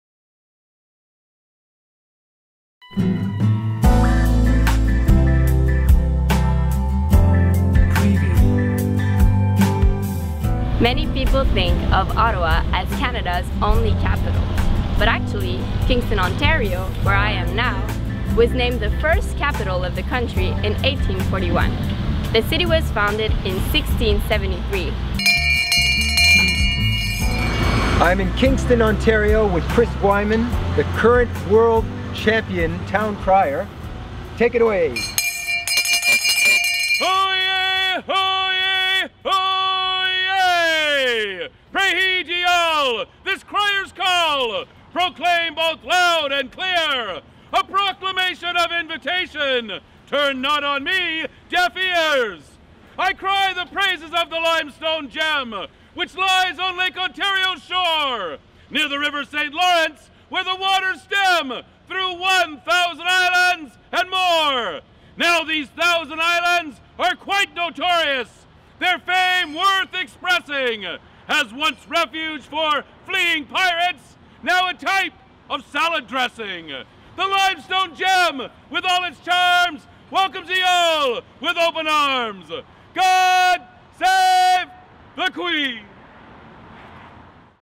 Where it all Began - Kingston, Ontario
Founded in 1673, and named Canada's first capitol in 1841 (yup that's right, our nation's first capitol was in Kingston, not Ottawa!), Kingston still retains some of that old-world charm but mixes it with a bustle of activity which makes it one of the greatest cities in Canada.

Kingston is known for its historic properties, as reflected in the city's motto "where history and innovation thrive". The municipality has nearly 700 properties listed in the heritage register, 20 National Historic Sites and in 2007, the Rideau Canal, along with the fortifications at Kingston, was designated a World Heritage Site, one of only 15 such sites in Canada.  All these places are responsible for the historic and architectural face of the city but it's the cities two universities, Queen's University and the Royal Military College of Canada (Canada's only military university), that give the place it's energy, enthusiasm and vigor!  Queens University currently has an enrollment of more than 13,000 undergraduate and 4,000 graduate students, which means there is no shortage of nightlife in the city!  But despite all this partying, Kingston still has the most PhD's per capita of any city in Canada.

The city is also famous for its fresh-water sailing, and hosted the sailing events for the 1976 Summer Olympics. Since 1972, Kingston has hosted more than 40 World and Olympic sailing championships.  And on another important sporting note Kingston lays claim to being the birthplace of ice hockey!

Kingston is in the heart of Canada located midway between Toronto and Montreal.  Paired up with a trip to one of Canada's other great cities or just as a destination onto itself Kingston is a great city to explore and enjoy.

There is a reason Kingston is still the "Heartbeat of Canada".